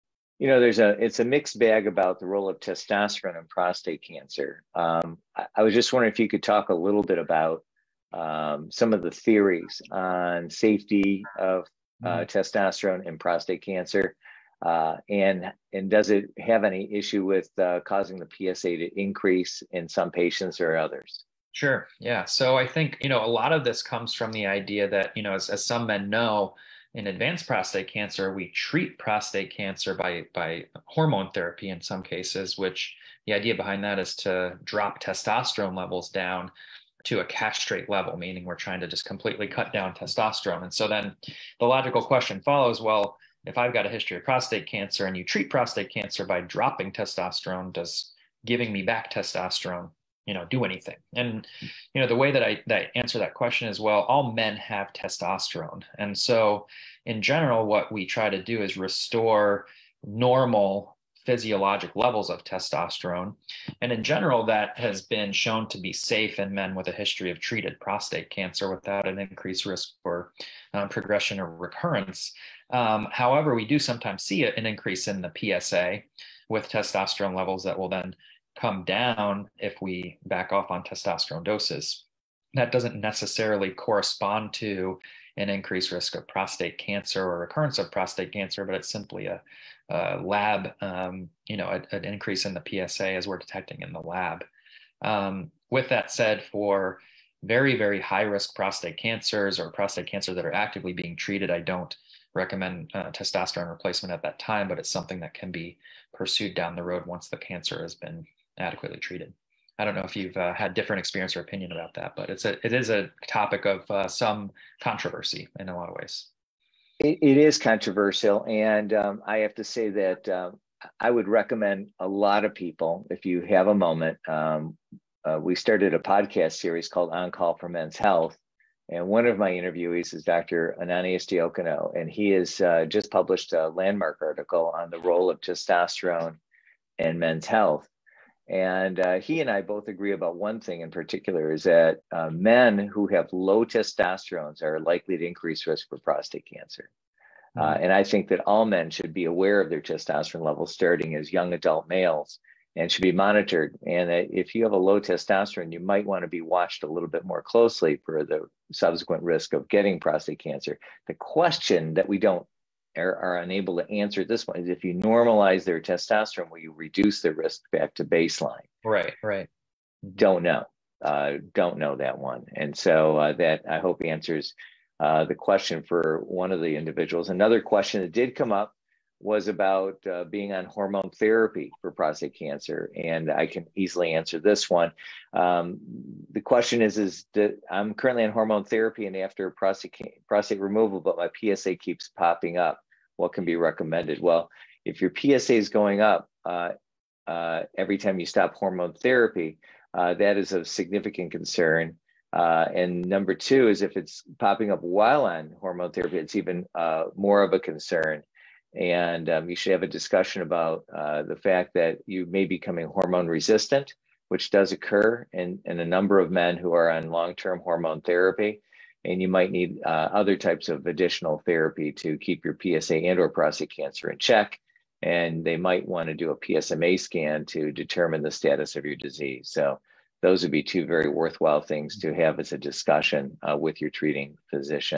The role of testosterone in prostate cancer
Dr. Jason Sandberg details the role that testosterone plays in prostate cancer in @MIMensHealthFoundation's Prostate Cancer Survivorship Series event "Sexuality After Prostate Cancer- A New Beginning".